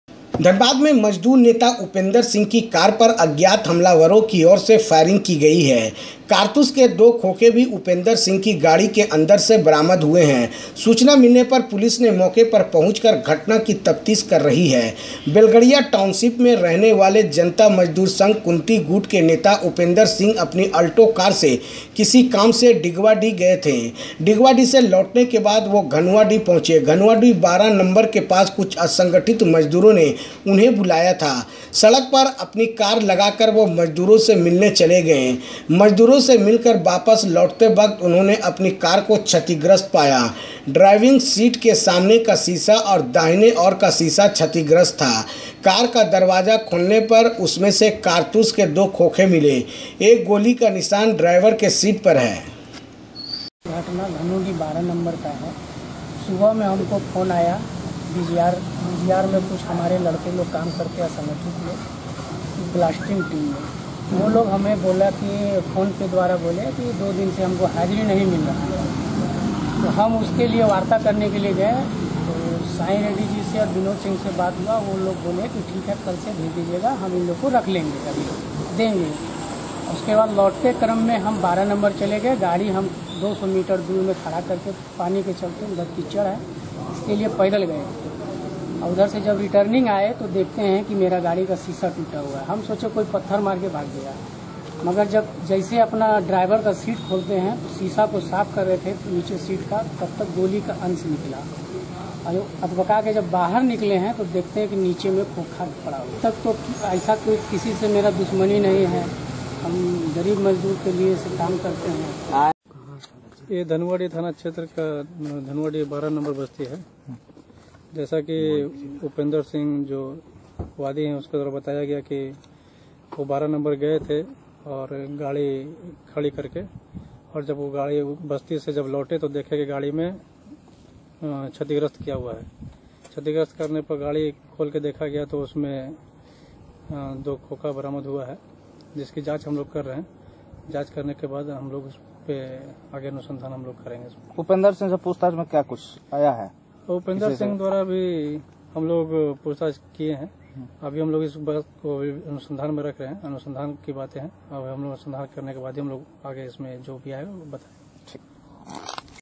AAJ KA SACH NEWS DHANBAD 27 . 09 . 2019 PHIR GOLI
धनबाद में  मजदूर नेता उपेंद्र सिंह की कार पर अज्ञात हमलावरों की ओर से फायरिंग की गई है. कारतूस के 2 खोखे भी उपेंद्र सिंह की गाड़ी के अंदर से बरामद हुए हैं. सूचना मिलने पर पुलिस ने मौके पर पहुंचकर घटना की तफ्तीश कर रही है. बेलगड़िया टाउनशिप में रहने वाले जनता मजदूर संघ कुंती गुट के नेता उपेंद्र सिंह अपनी ऑल्टो कार से किसी काम से डिगवाडीह गए थे. डिगवाडीह से लौटने के बाद वह घनुडीह पहुंचे. घनुडीह 12 नंबर के पास कुछ असंगठित मजदूरों ने उन्हें बुलाया था. सड़क पर अपनी कार लगाकर वह मजदूरों से मिलने चले गए. मजदूरों से मिलकर वापस लौटतो वक्त उन्होंने अपनी कार को क्षतिग्रस्त पाया. ड्राइविंग सीट के सामने का शीशा और दाहिनी ओर का शीशा क्षतिग्रस्त था. कार का दरवाजा खोलने पर उसमें से कारतूस के 2 खोखे मिले. एक गोली का निशान ड्राइवर की सीट पर है.